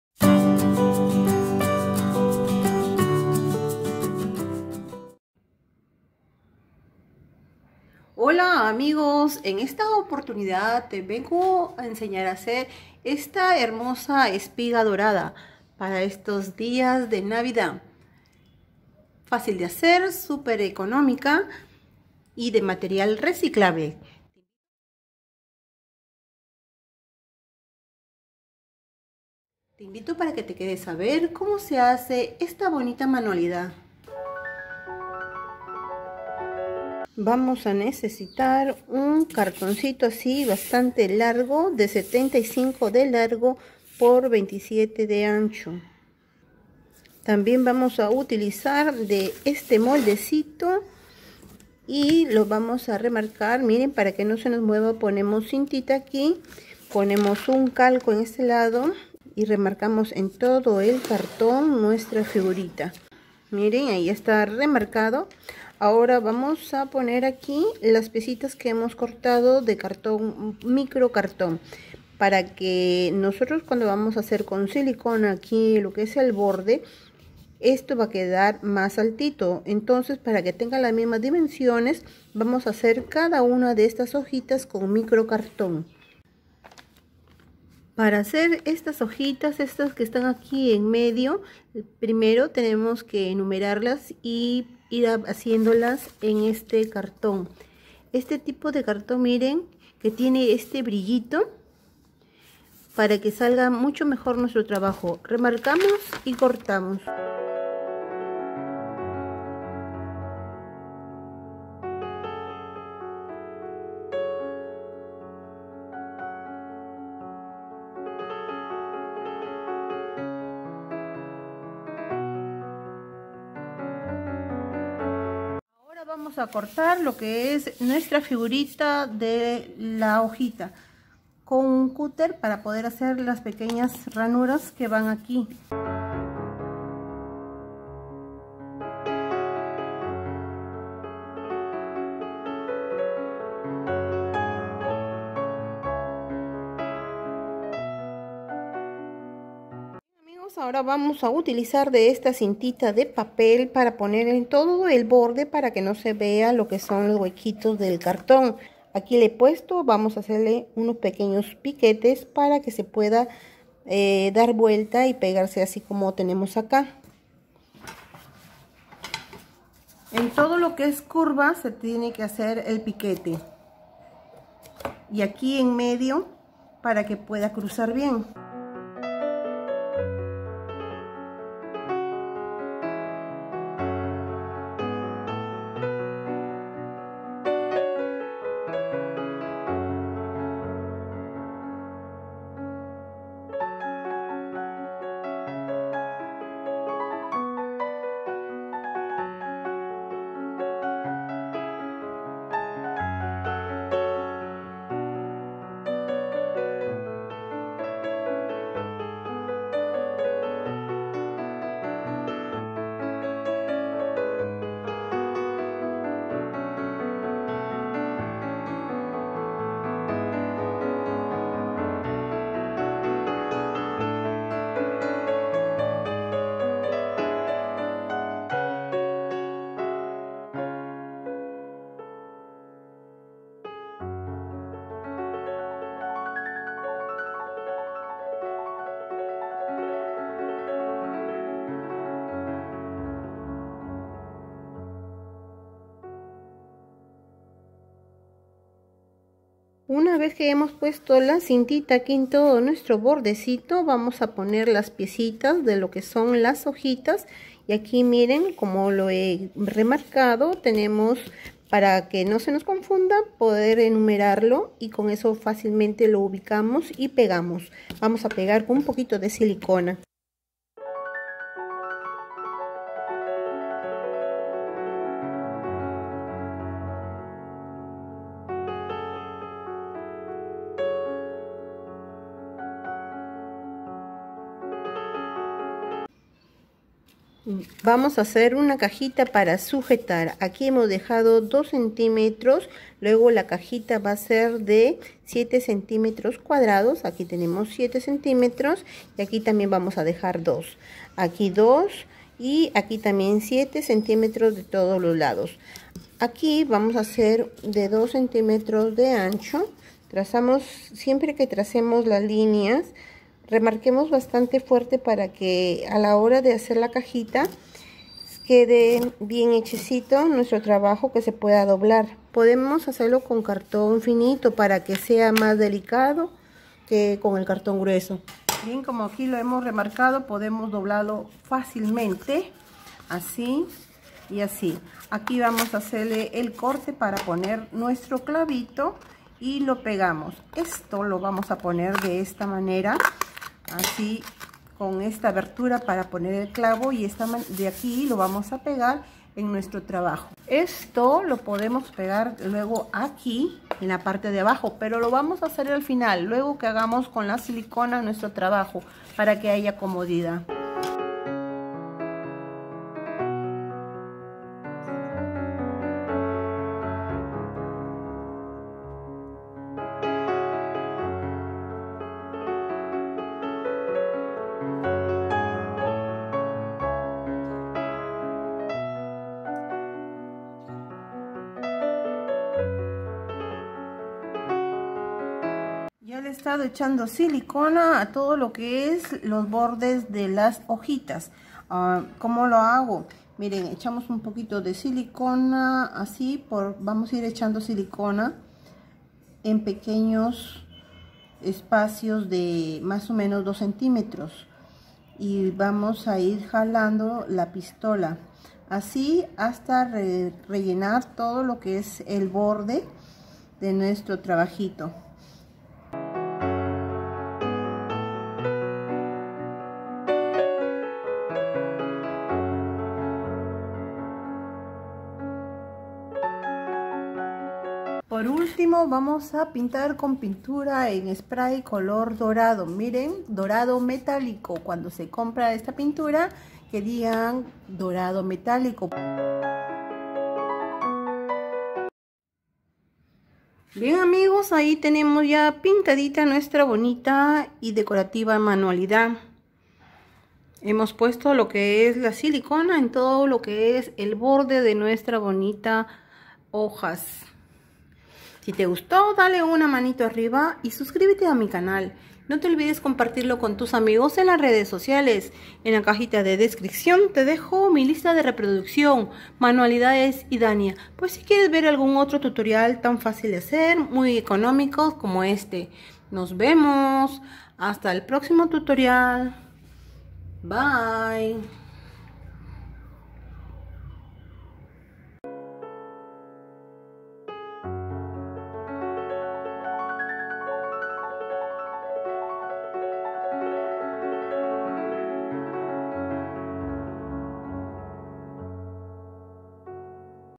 Espiga decorativa dorada, Adorno de pared, como hacer un adorno reciclado.
Este bonito adorno está echo con material reciclado, fácil y económico, espero te guste. Te dejo el enlace de mi lista de reproducción: